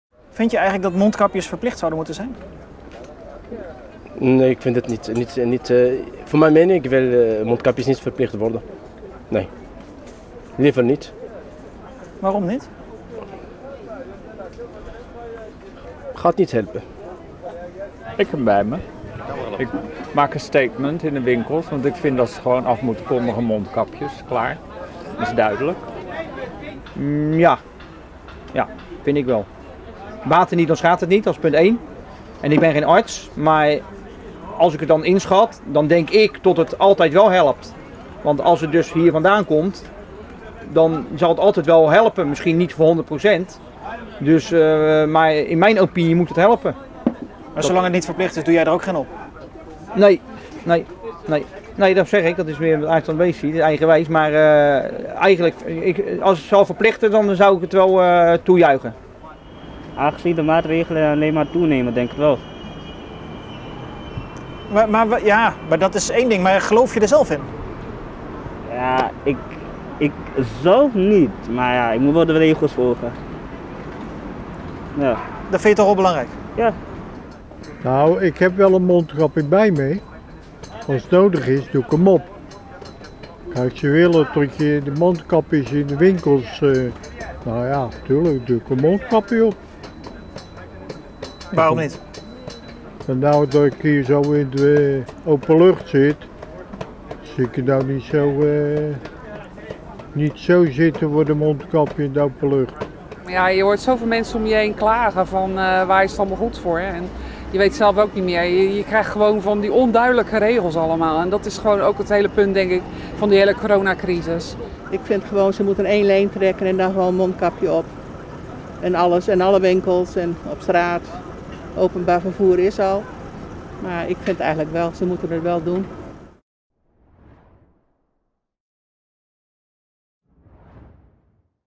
Gedwongen gezichtsbescherming: willen Rotterdammers een mondkapjesplicht?
Kabinet Rutte maakte woensdag bekend dat zij Nederlanders ‘dringend adviseren’ om overal mondkapjes te dragen in publieke binnenruimtes. Komende vrijdag wordt er meer duidelijk of er een plicht komt. In Rotterdam maakte de verplichte mondkapjeszones stilletjes de aftocht na een paar weken. Hoe kijken Rotterdammers naar het verplichte mondkapje? Dragen zij ‘m al, en waarom wel of niet?We vroegen de marktbezoekers en -verkopers naar hun mondkapjesgewoonten en of zij heil zien in een verplichting.[iframe Een terugblik en hoe nu verder…Vrijdag 2 oktober, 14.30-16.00 uur, TV Rijnmond: Lof der Geneeskunst.Een talkshow met Erasmus MC deskundigen Diederik Gommers, Marion Koopmans en Ernst Kuipers.